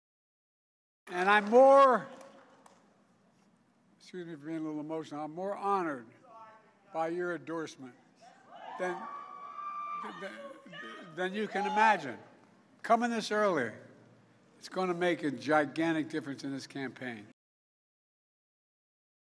Biden kicks off re-election bid
President Biden holds kick off rally for his re-election bid in Philadelphia and wins endorsement of AFL-CIO. CNN video.